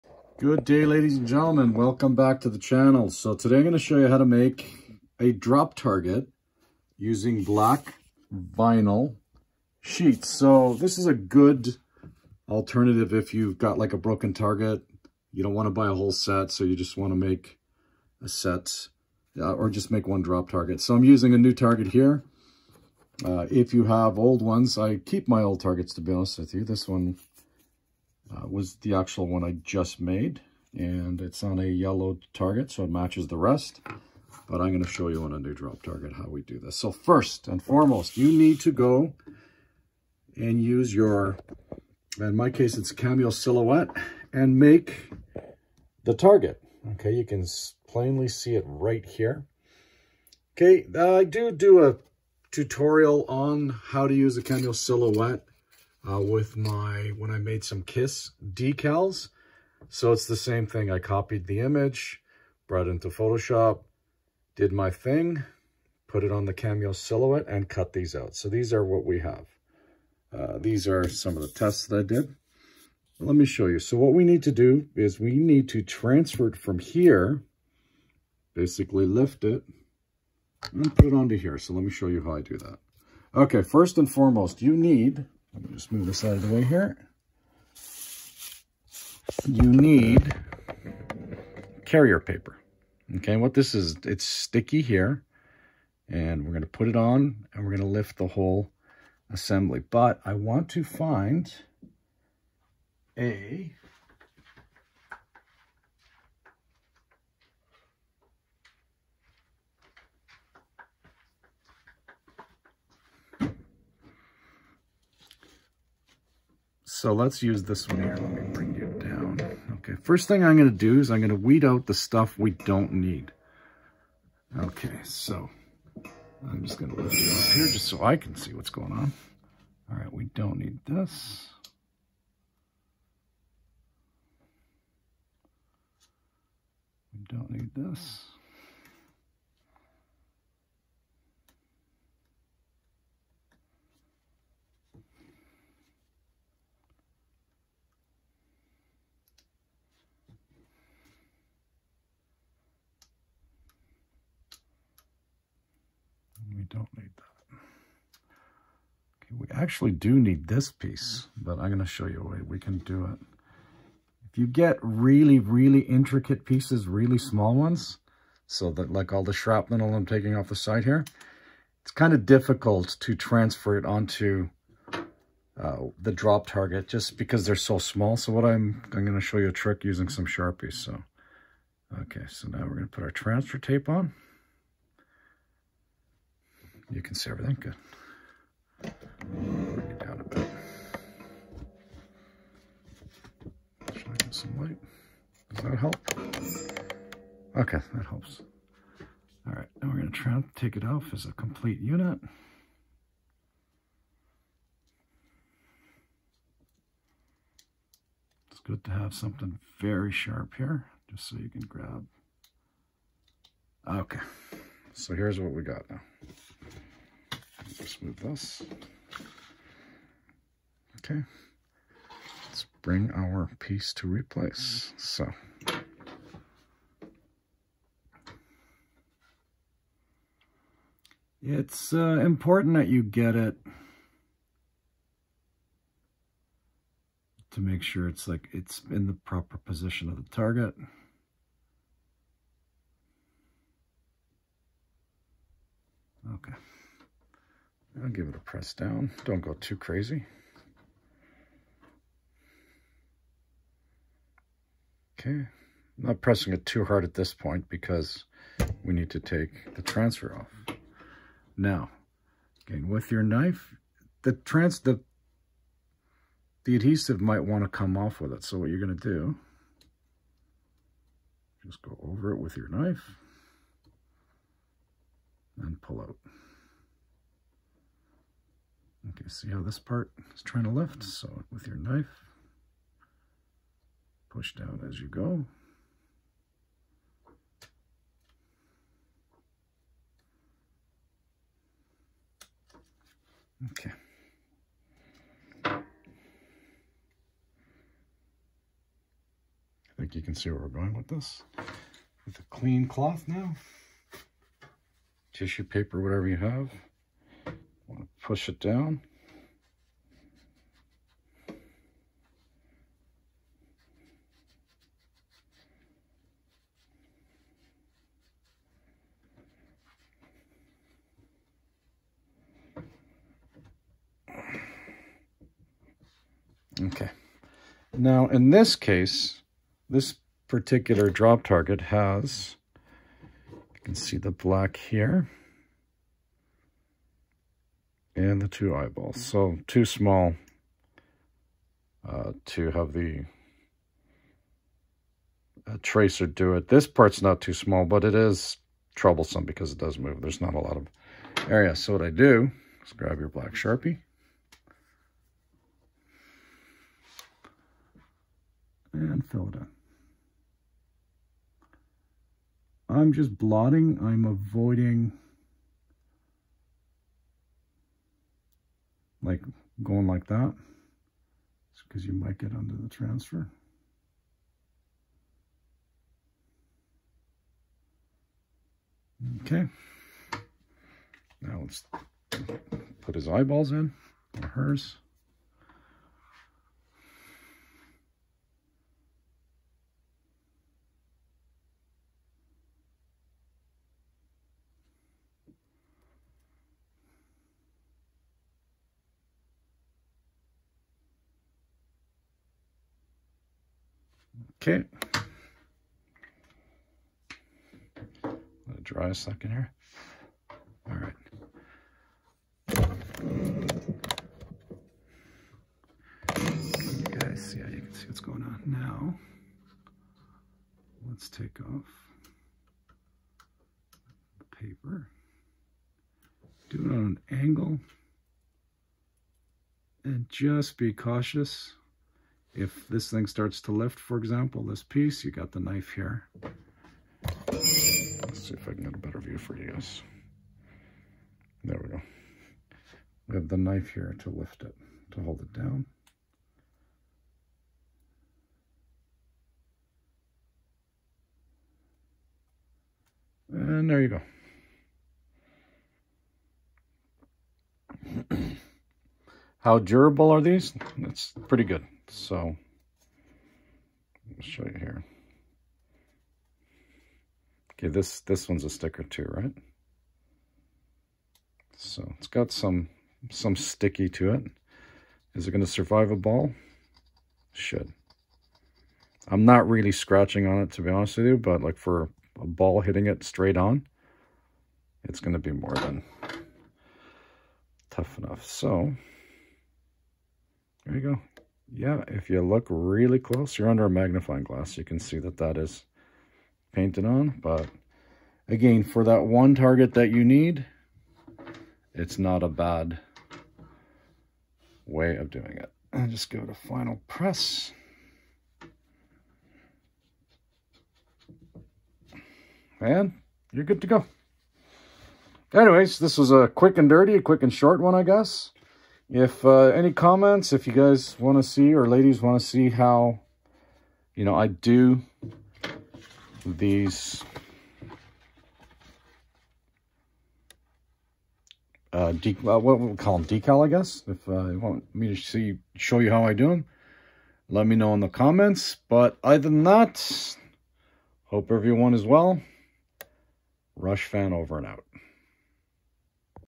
Pinball Machine drop target restoration on the cheap
Let reface a pinball drop target with a Cameo Silhouette cutter and some cheap black vinyl.  Perfect for replacing that one target that breaks and not having to buy a complete set